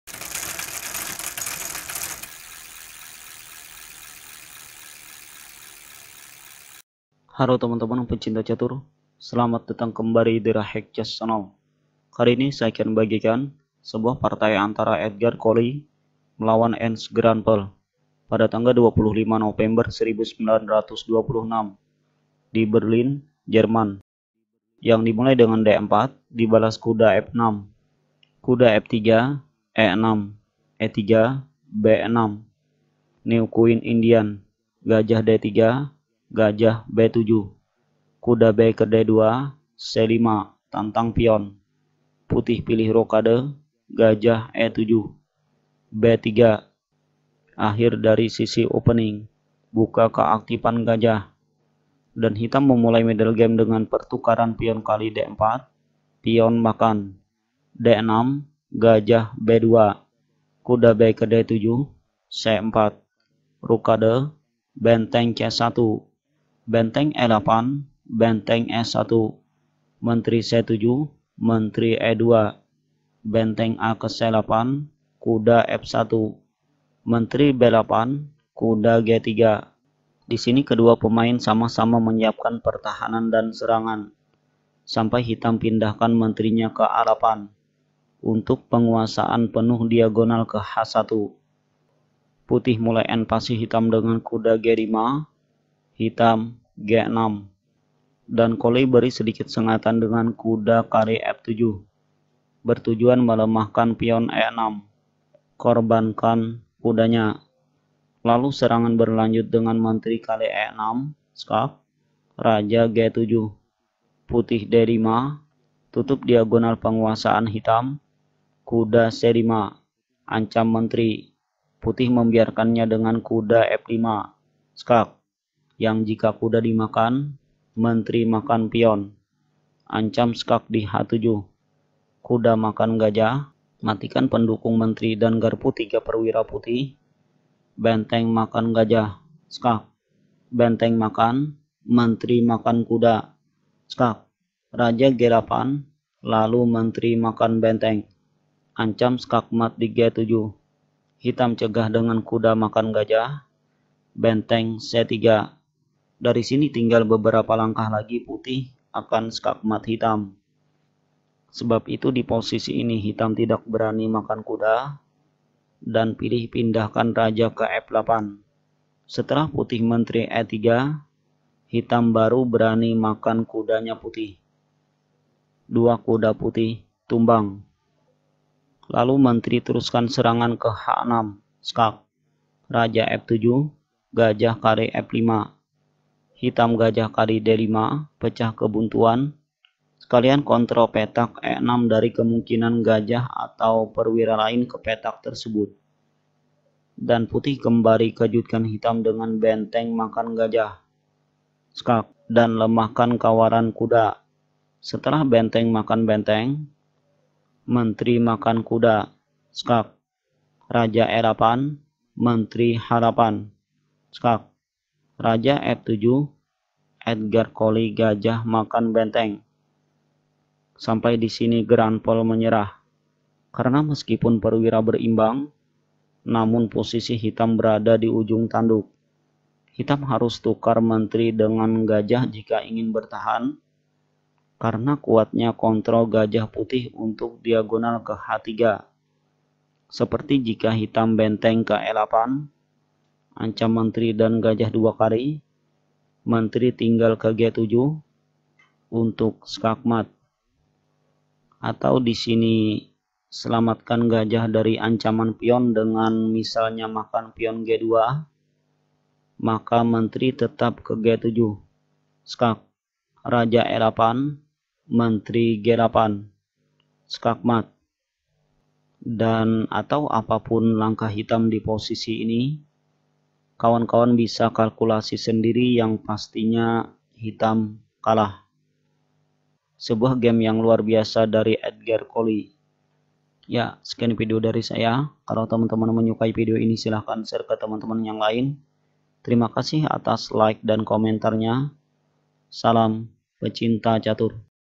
KALKULASI MASA DEPAN | Edgar Colle VS Ernst Gruenfeld
Halo kawan-kawan pecinta catur, kali ini saya akan membagikan sebuah partai antara Edgar Colle VS Ernst Gruenfeld pada tanggal 25 November 1926 di Berlin, Jerman.
Indian Game: Yusupov-Rubinstein System (A46)
1. d4 Nf6 2. Nf3 e6 3. e3 b6 4. Bd3 Bb7 5. Nbd2 c5 6. O-O Be7 7. b3 cxd4 8. exd4 d6 9. Bb2 Nbd7 10. c4 O-O 11. Rc1 Re8 12. Re1 Qc7 13. Qe2 Rac8 14. Nf1 Qb8 15. Ng3 Qa8 16. Ng5 g6 17. Nxf7 Kxf7 18. Qxe6+ Kg7 19. d5 Nc5 20. Nf5+ Kf8 21. Qe3 gxf5 22. Qh6+ Kf7 23. Bxf5 Bxd5 24. Rxe7+ Rxe7 25. Qxf6+ Ke8 26. Qh8+ Kf7 27. Bxc8
1-0